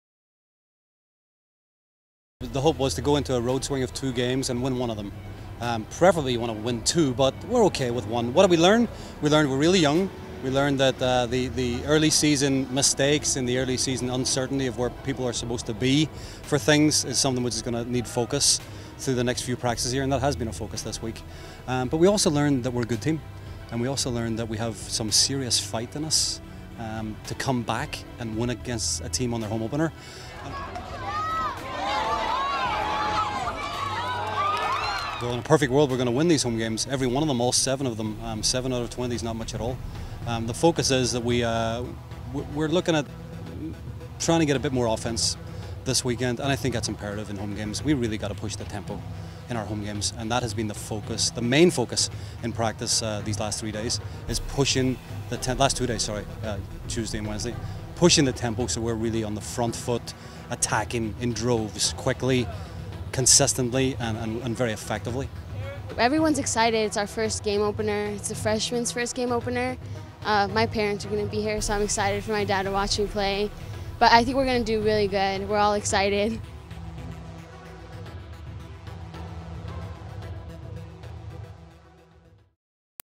UNLV Women's Soccer - August 28, 2013
UNLV Athletics catches up with the women's soccer team after their first weekend of action this fall.